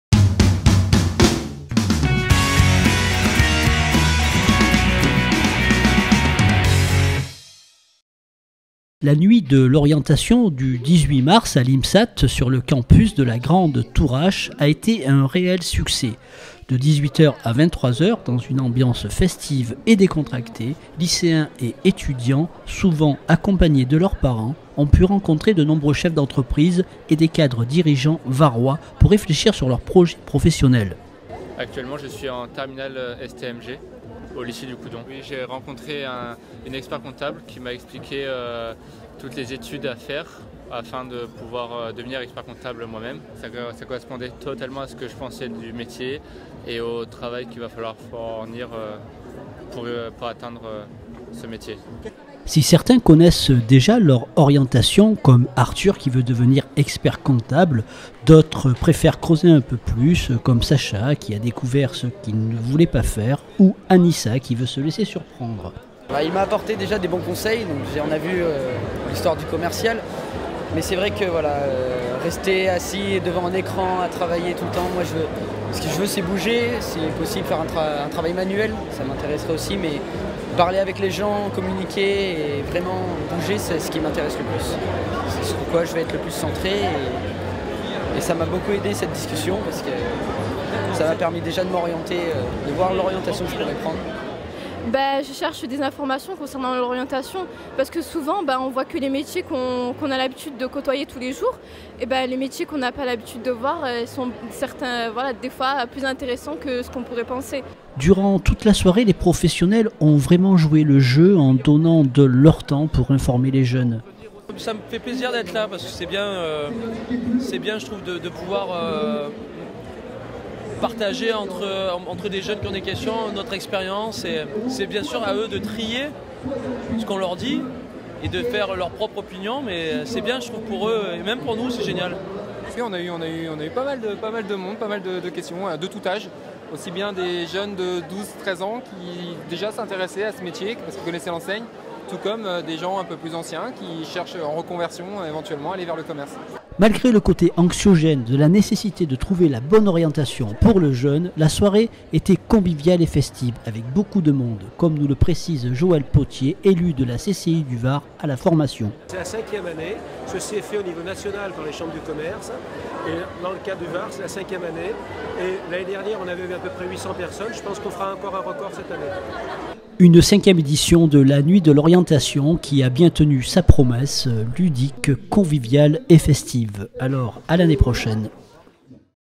Nuit de l'orientation 2016, à l'IMSAT La Garde
Belle soirée à l'IMSAT de La Grande Tourrache, pour la 5è édition de la Nuit de l'Orientation.